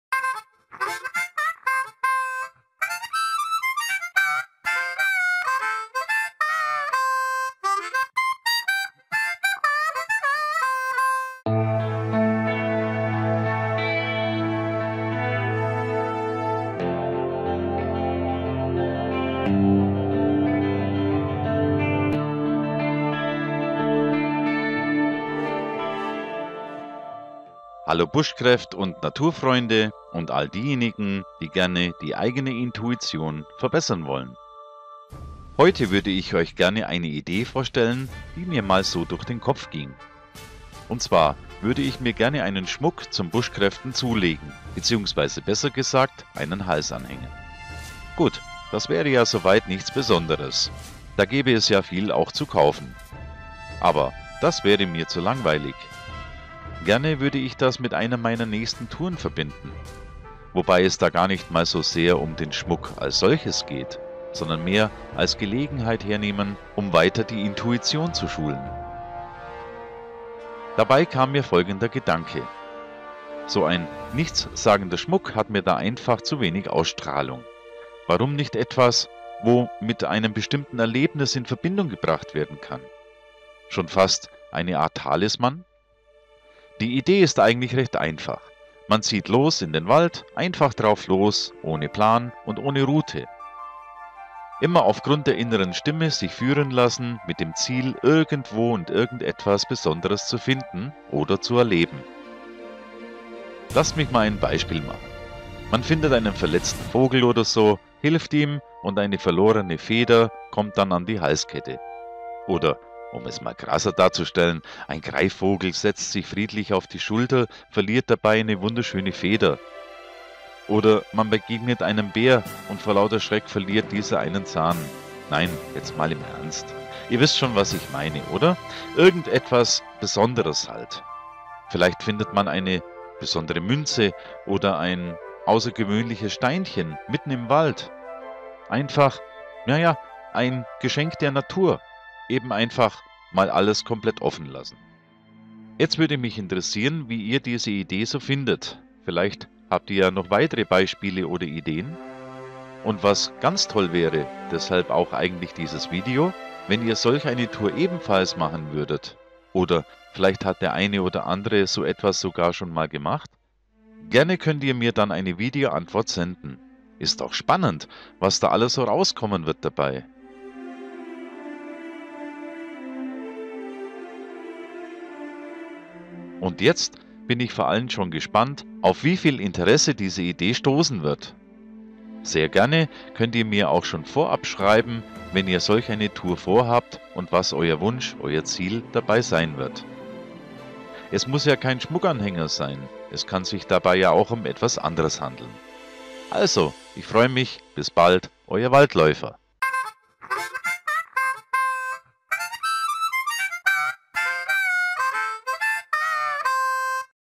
Bushcraft: Intuition - Schmuck, Challenge
Mach mit! Die Intuition zeigt dir den Weg!
Freue mich auf Videoantworten...
Da ging es darum, einen schönen Grillplatz zu finden, und ich hatte mehr bekommen als ich erwartet hatte.